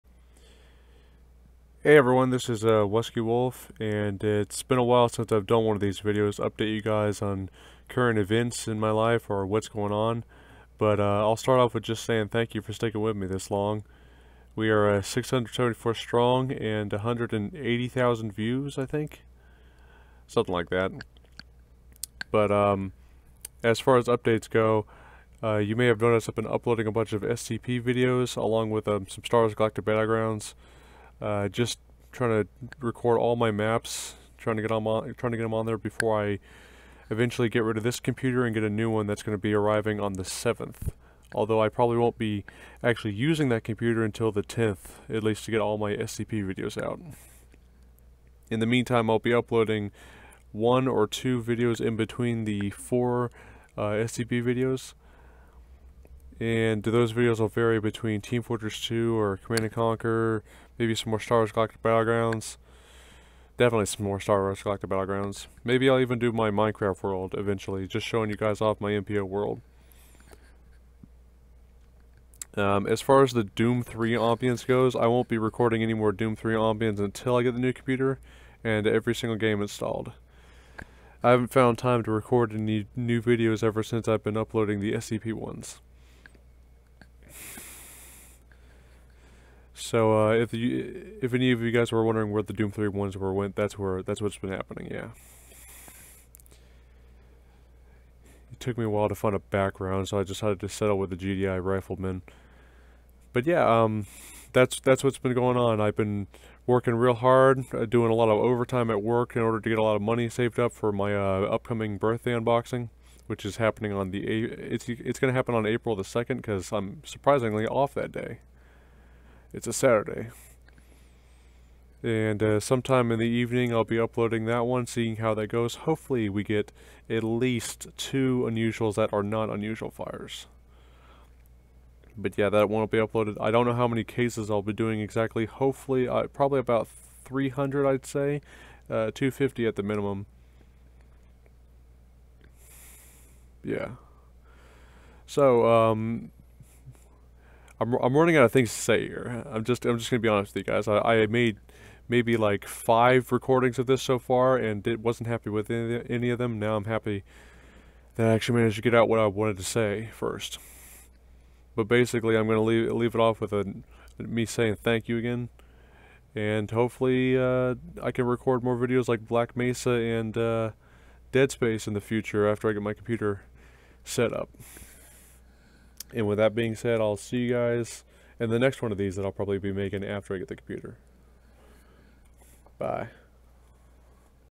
The Update video! (Current events and me being nervous on video)
An update of sorts, telling you guys whats been up with the Scpsl spam, lack of Doom 3 ambience, soon to be birthday unboxing, and some other stuff.

Sorry if things seem a little awkward during the video I haven't done one of these in a few years so i'm a little nervous and well rusty hehe.

Anyway i'll make another one of these soon before or after I get the computer!